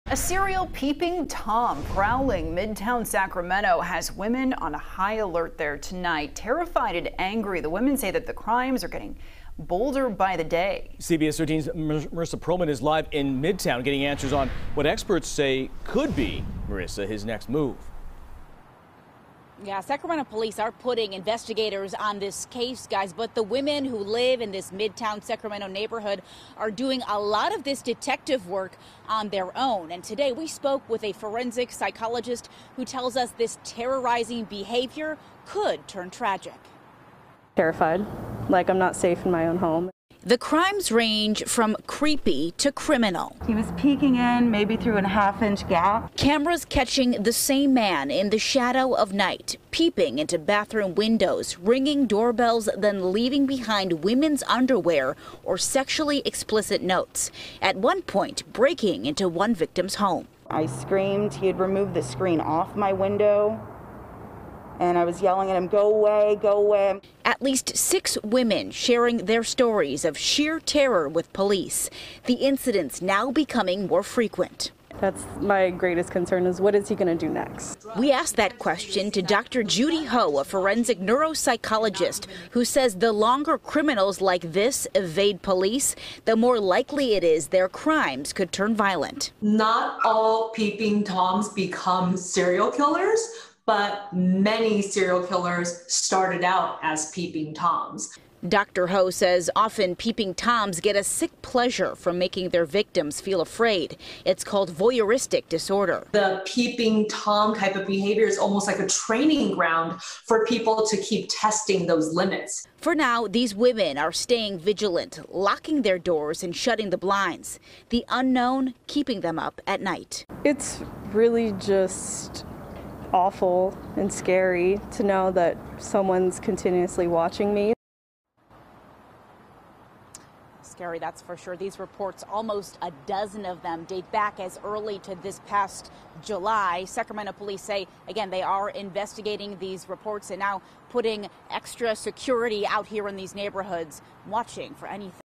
Downtown Prowler On The Loose In Sacramento
Multiple women have reported the same man lurking at night in their midtown neighborhood. The crimes are becoming increasingly more chilling.